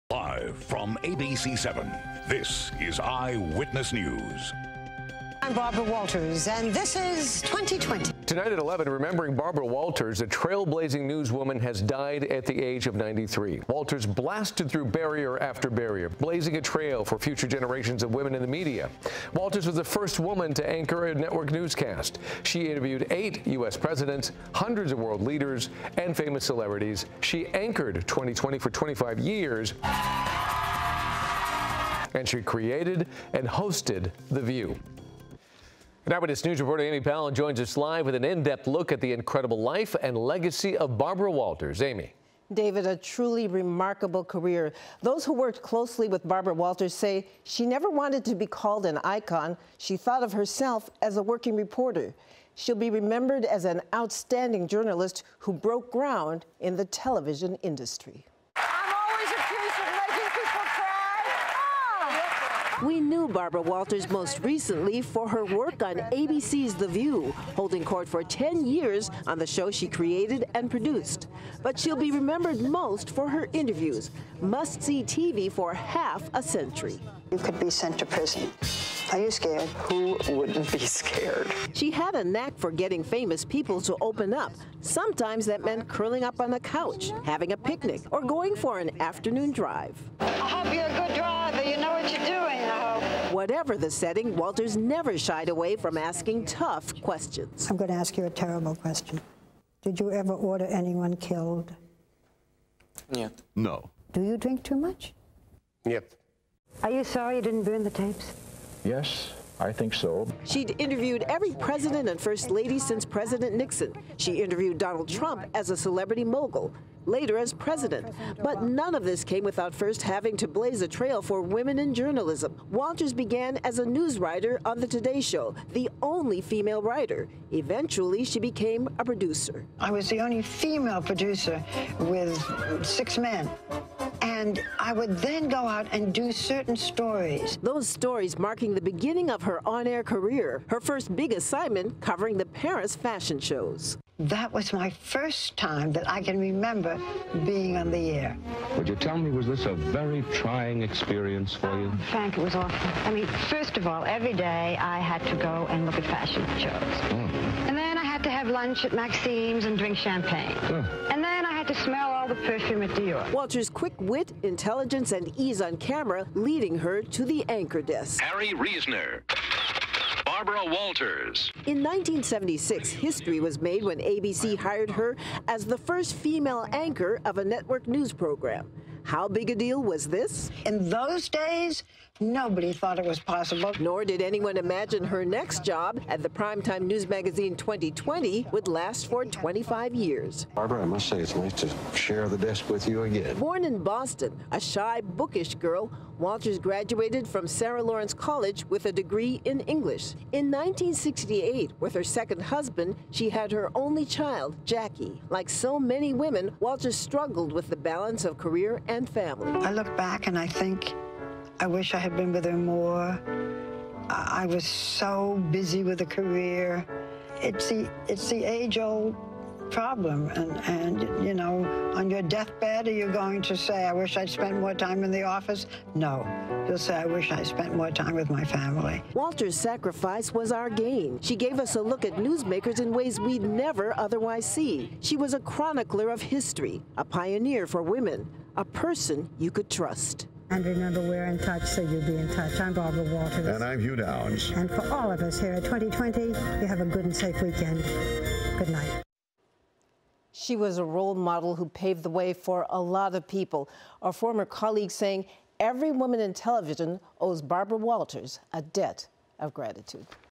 Remembering Barbara Walters
Remembering the icon, legend, trailblazer Barbara Walters. She paved the way for so many - we learned from her - and remain in awe of her to this day.